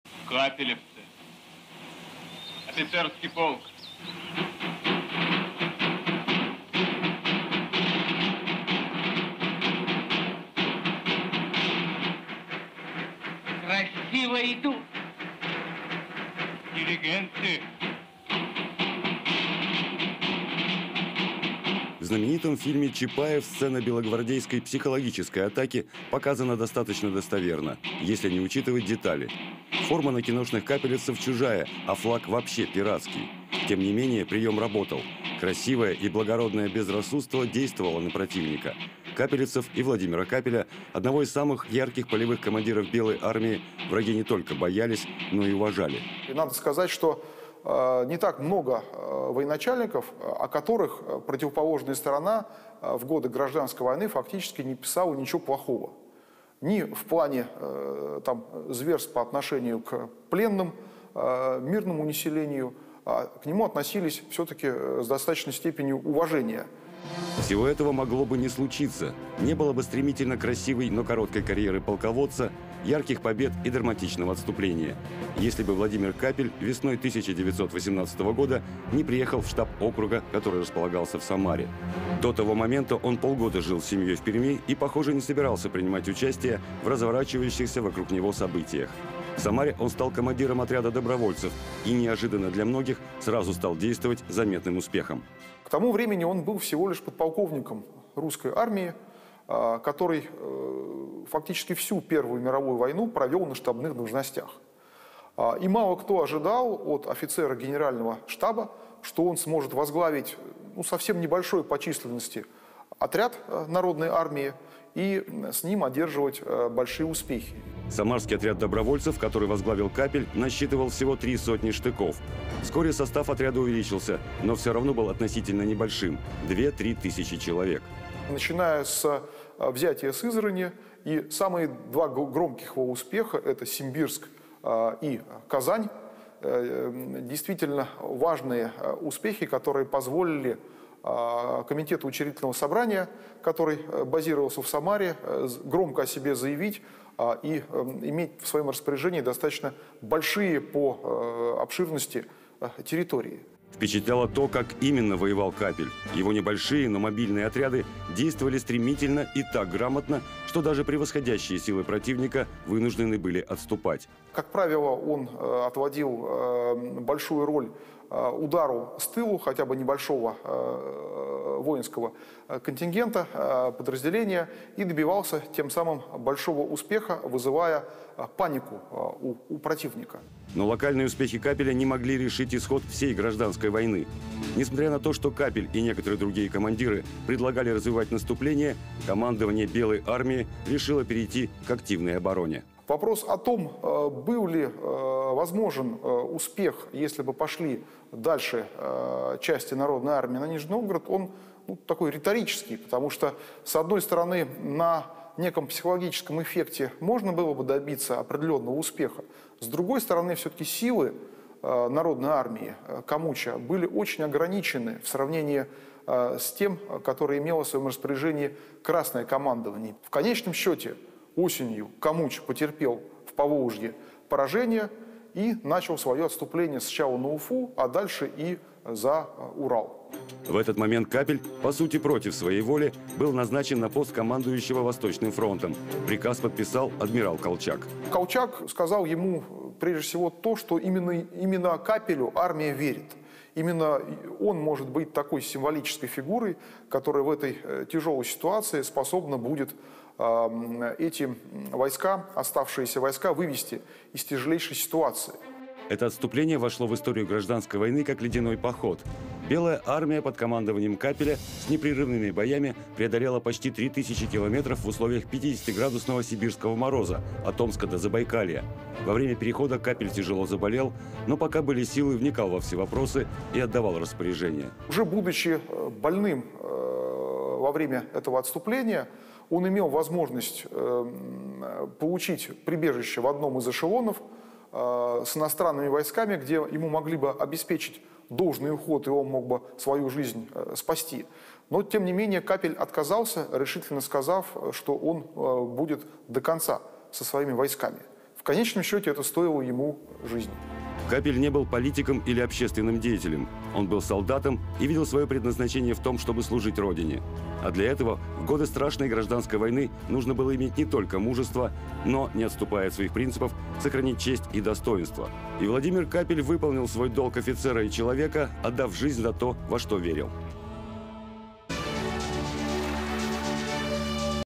Белый рыцарь: Владимир Каппель
Его жизнь была короткой, но яркой и насыщенной событиями. Он был грамотным и бесстрашным офицером, добивался побед не числом, а умением, и, если бы не революция, его могла ждать блестящая военная, а может быть, и политическая карьера. Монархист, ярый противник большевиков, «белый лебедь контрреволюции» Владимир Каппель родился 28 апреля 1883 года,140 лет назад. Сегодня поговорим о том, как сложилась судьба одного из самых заметных участников Белого движения.